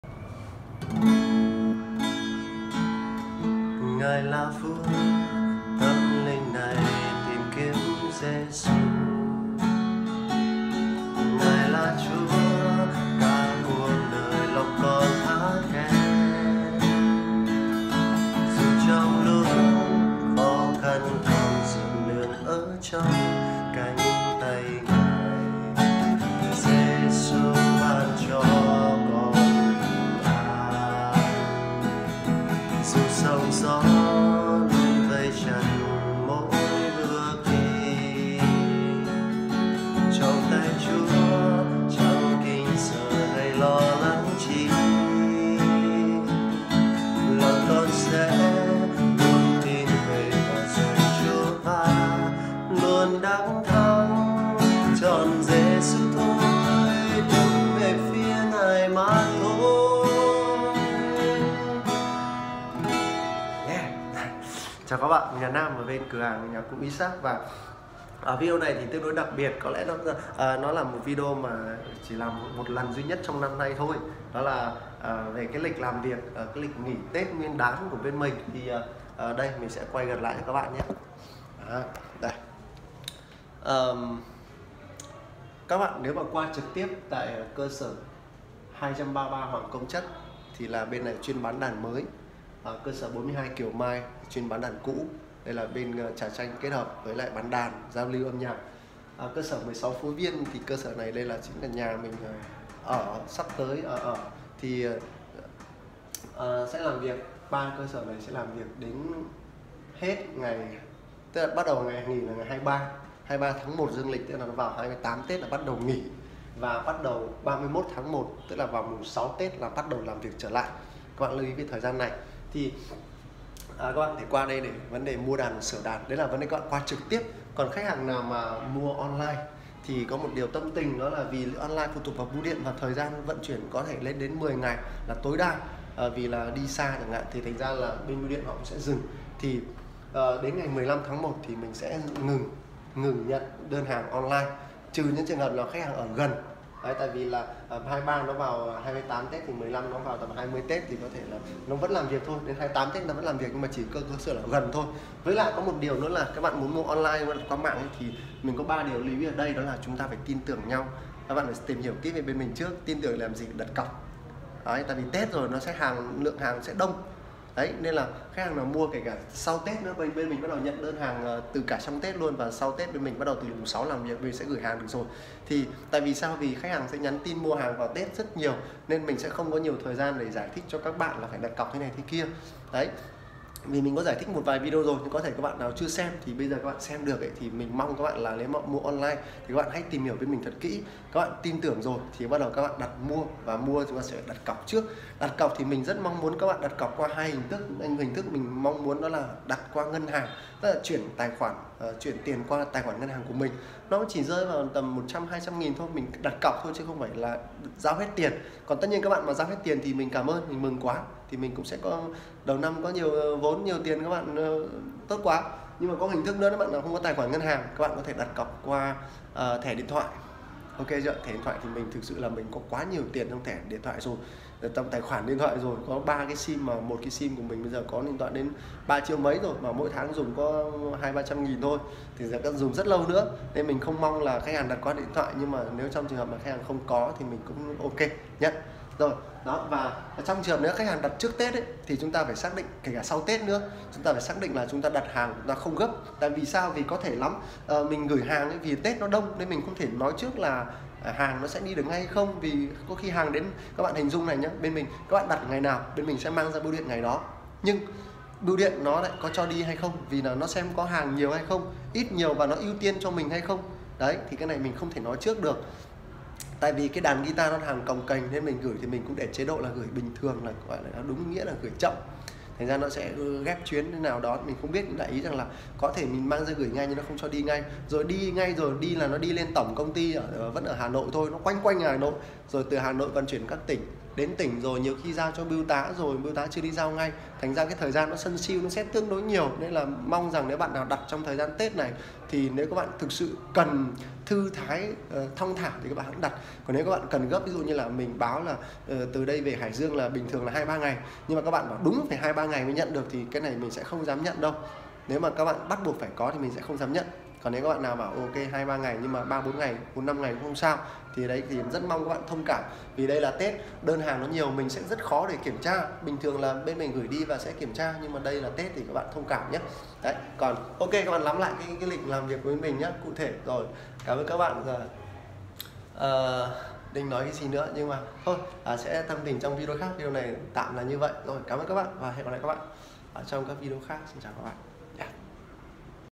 Lịch nghỉ Tết và 1 vài lời tâm tình với khách mua hàng dịp Tết 2020
Chào các bạn! Mình là Nam.
Trong video này mình sẽ thông báo về lịch nghỉ Tết 2020
Và 1 vài lời tâm tình dành cho khách hàng mua hàng trên mạng tại Guitar isaac!
Lịch nghỉ Tết tại cơ sở trực tiếp 23/01-31/01 (Tức là 28 Tết bắt đầu nghỉ. Mùng 06 Tết sẽ bán hàng trở lại)
Khách mua hàng trên mạng sẽ ngừng nhận đơn từ ngày 15/01 đối với khách hàng ở xa.
Và ngày 20/01 đối với khách hàng ở gần.
Quý khách lưu ý sẽ đặt cọc trước nhé!